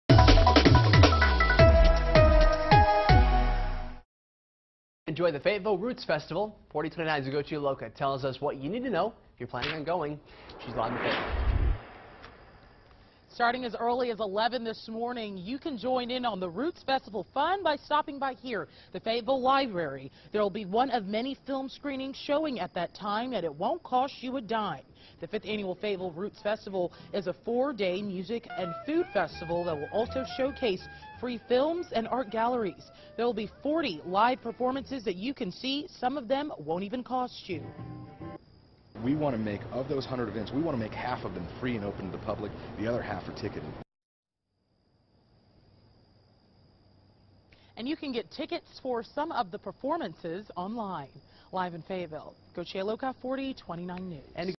Fayetteville's roots festival kicks off today
40/29's Ugochi Iloka is in Fayetteville where the Roots Festival kicks off this morning.